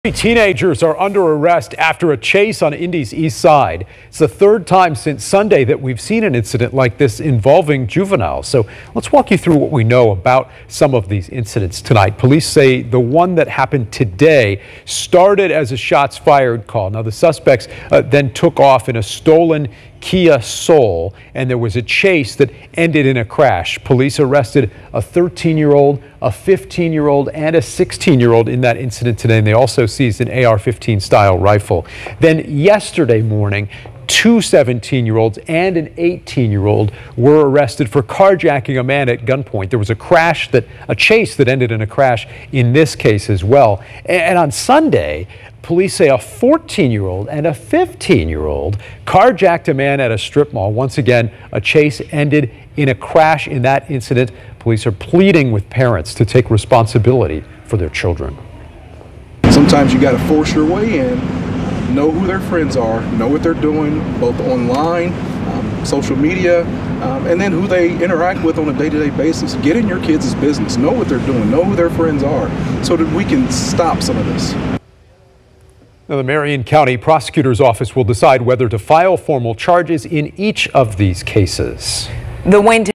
3 teens arrested after early morning chase involving stolen Kia
3 teens arrested after early morning chase involving stolen Kia